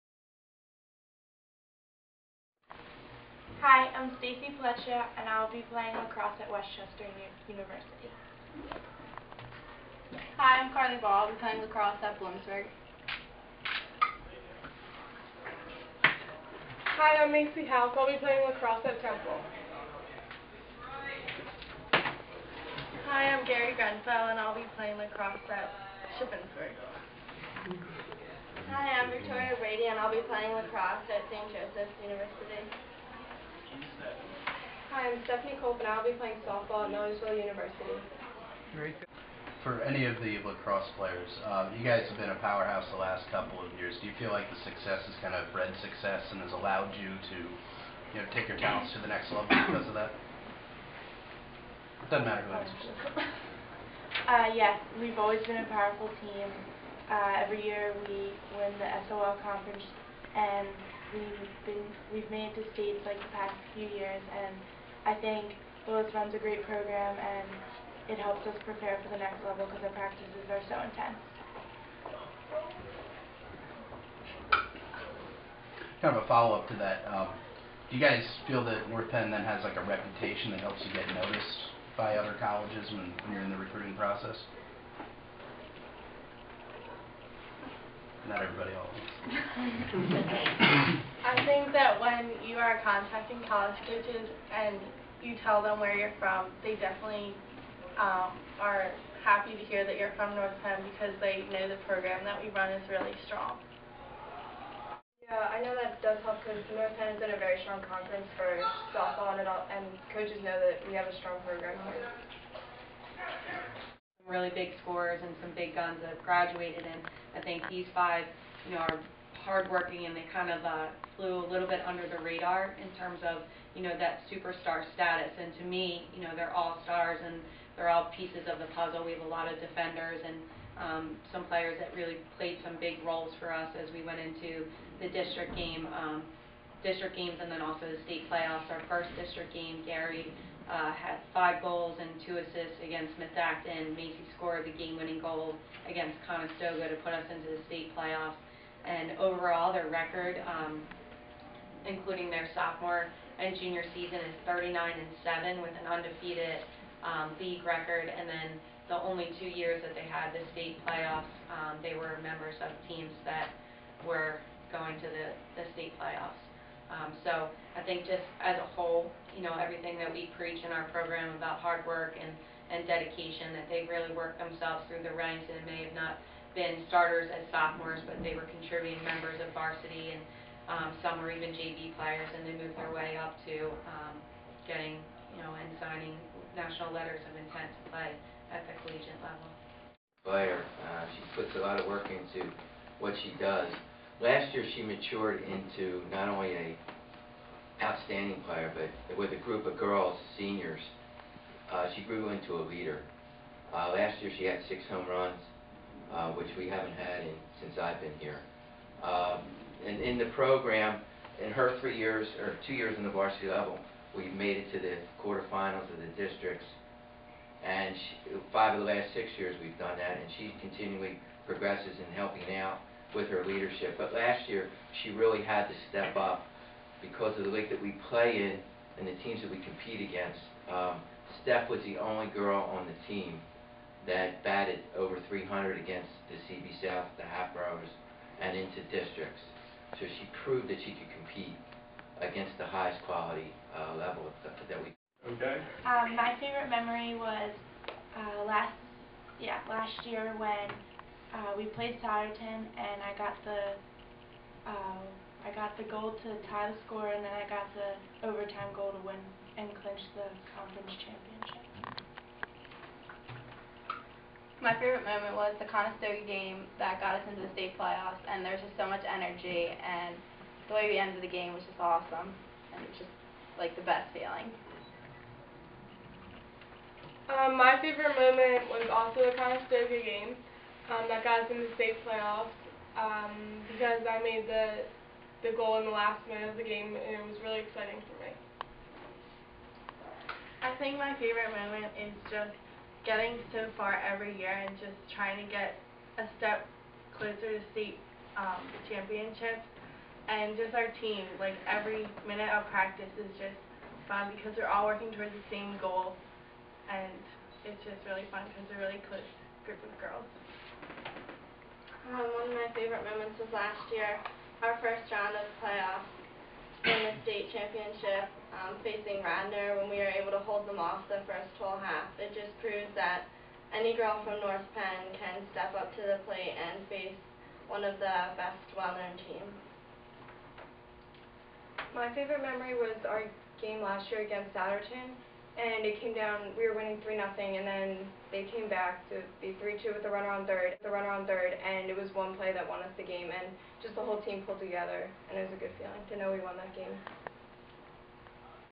NP Signings 11 17 10
Five lacrosse players and one softball player sign their National Letters of Intent